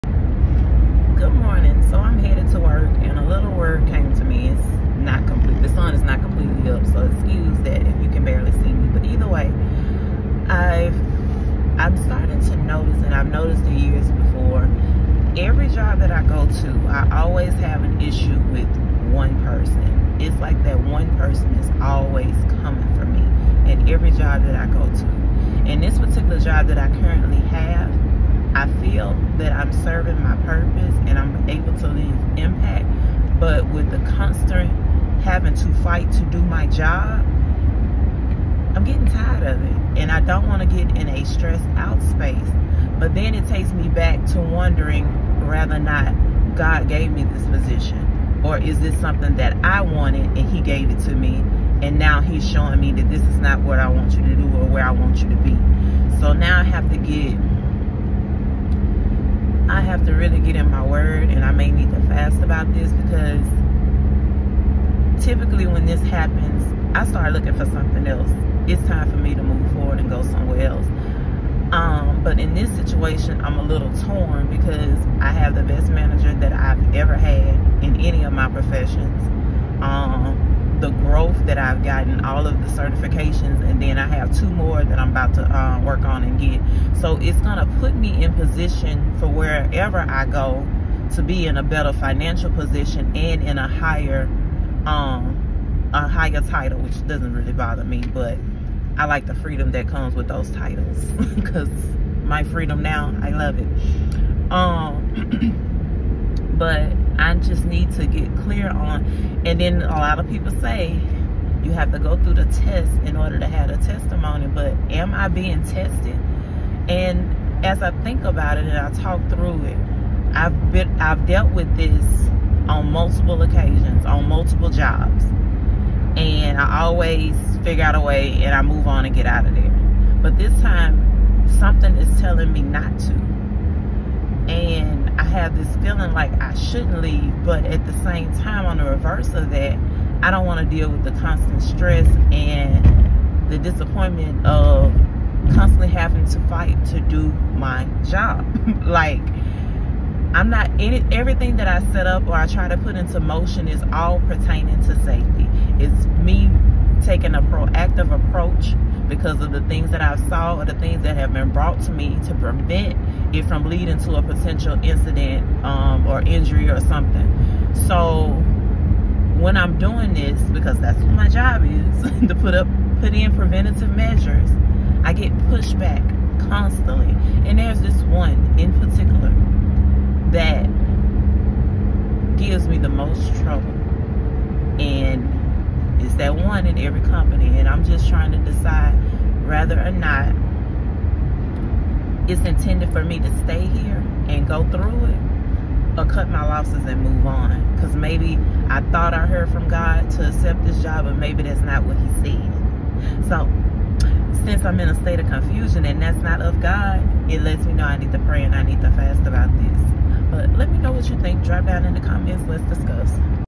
Overcoming Workplace Doubt: Staying Confident in Your Calling
I've been praying about whether I should stay at my current job or start looking for a new position. I'm questioning if God truly called me to this role and company or if it's time to move on.

While certain people advise me to stick it out longer, other voices make me doubt if this job is right for my calling. Their conflicting perspectives have me feeling torn about what direction is best.

In this faith-focused video, I share my struggle with deciphering God's will in navigating my career.

I also discuss how I'm working to quiet self-doubt, tune out the opinions of others, and listen to the Holy Spirit. My hope is that by candidly sharing my own discernment process, it will help anyone else questioning "Is this the job God wants me to have?"

If you can relate to seeking direction for your career path, I hope my story provides some guidance and reassurance. Let's pray for God's purpose together as we move forward in our work.

Access to my Business Formation Checklist

Printable: Being Updated
Digital with Fillable spaces: Being Updated

Equipment Used:

Mini Ring Light